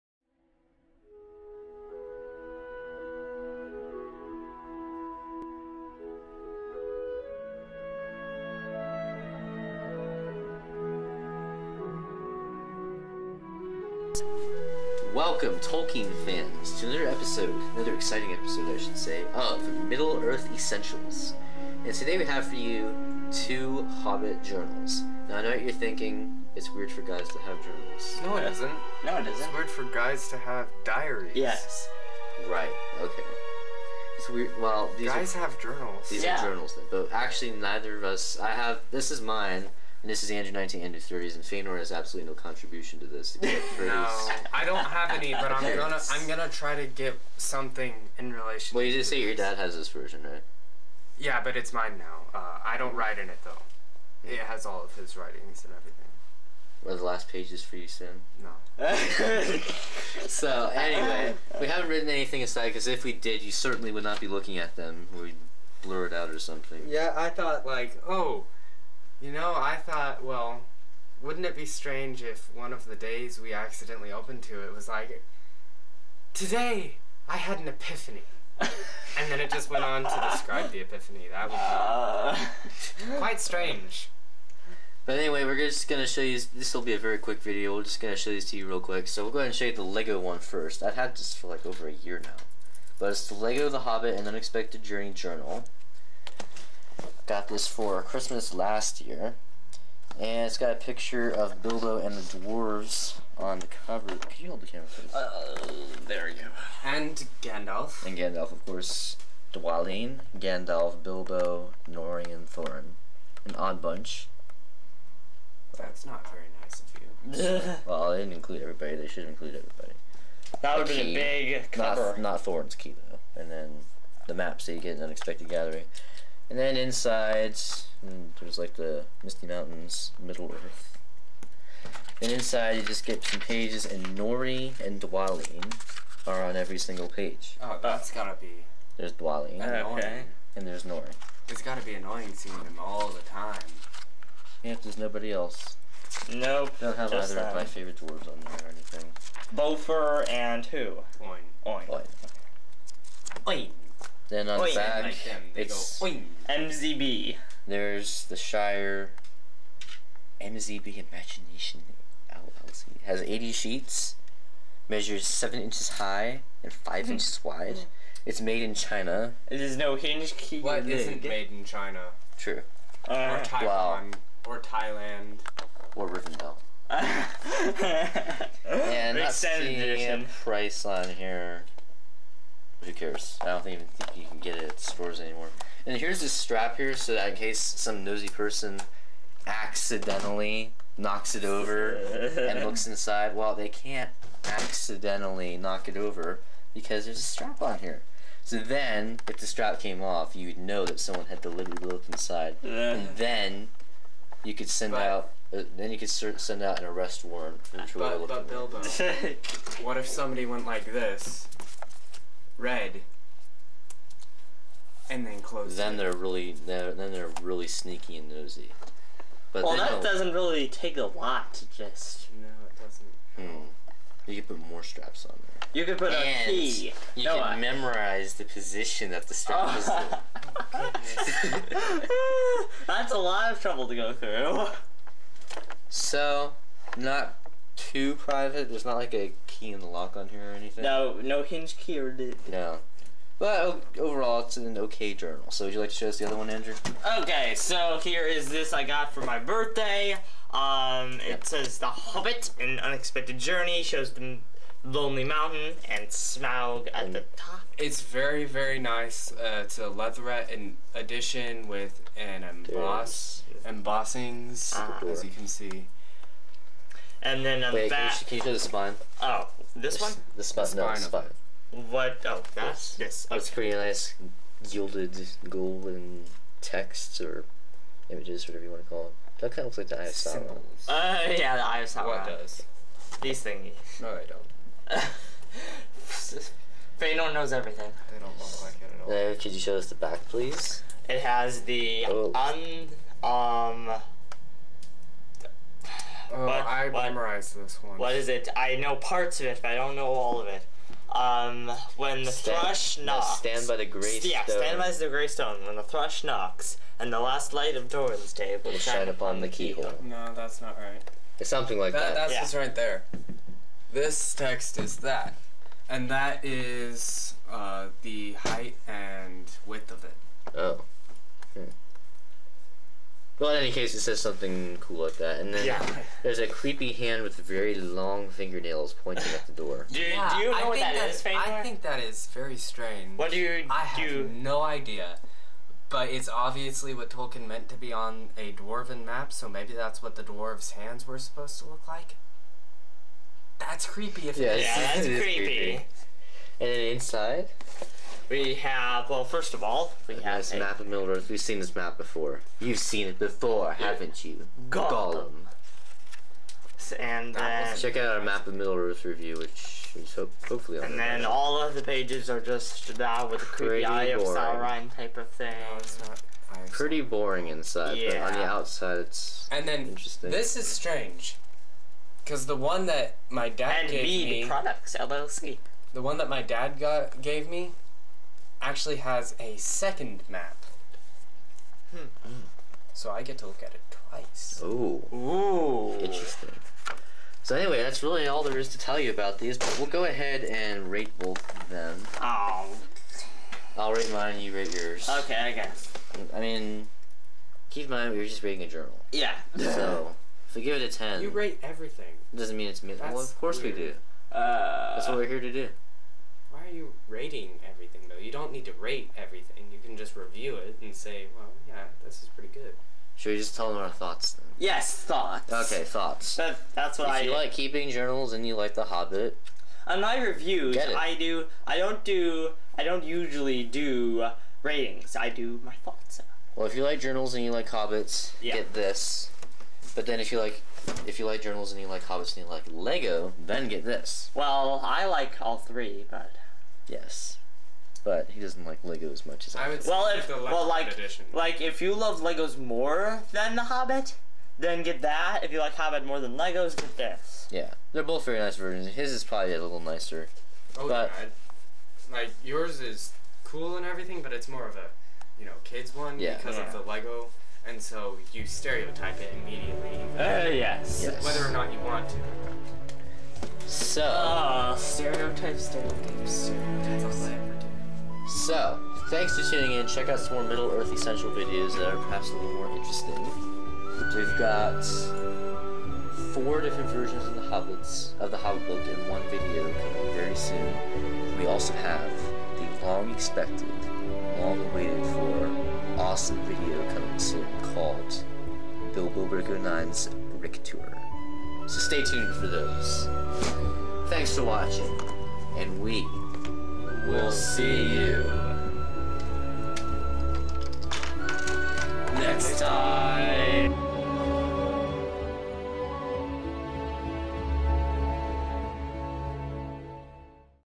Hobbit Journals Comparison
In this short video, Feanor, andrew, and I showcase two different Hobbit journals.